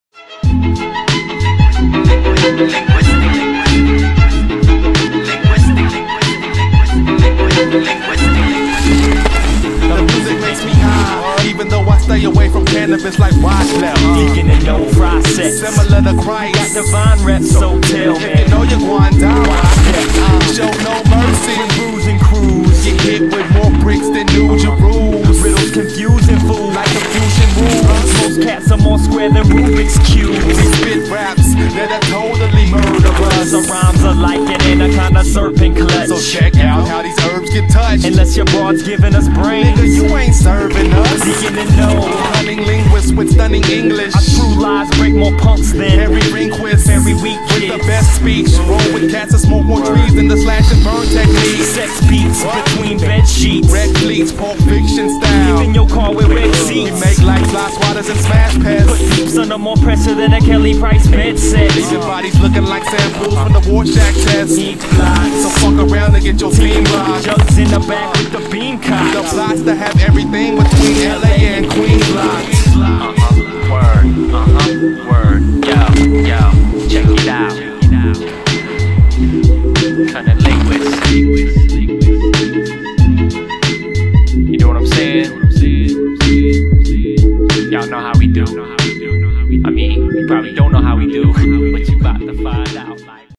James Withey - Morley woods winter edit 2018 (transistion tr11)
James withey turns it up to send on his local trails!